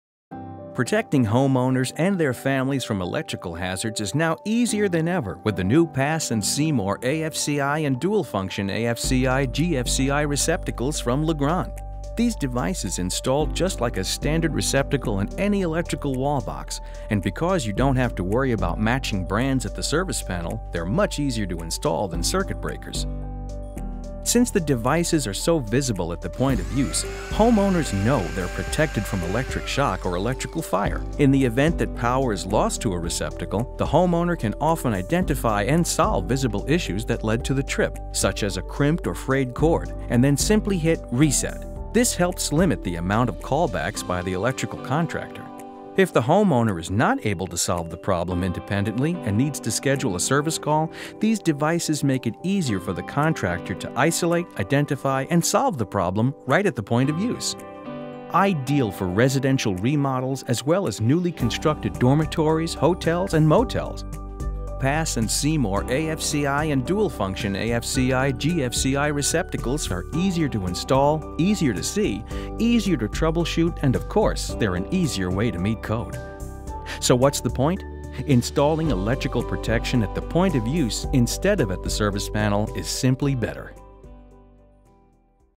Let's Get to the Point
Pass & Seymour Outlet Branch Circuit Arc-Fault Circuit Interrupter (AFCI) Receptacles and Dual-Function AFCI/GFCI Receptacles are the latest innovations in electrical safety technology, and a Code-approved, cost-effective alternative to AFCI and Dual-Function AFCI/GFCI breakers.